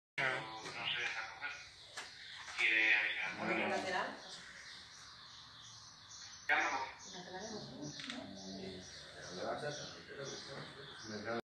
Huge Python Falls From Flat And Shocks Young Mum
This is the moment Spanish cops hold up a huge python after it reportedly fell from a fifth-floor apartment into an interior courtyard where it shocked a young mum.

Daniela, who has a six-year-old child and a toddler, saw the enormous snake when she opened the window to let in some air.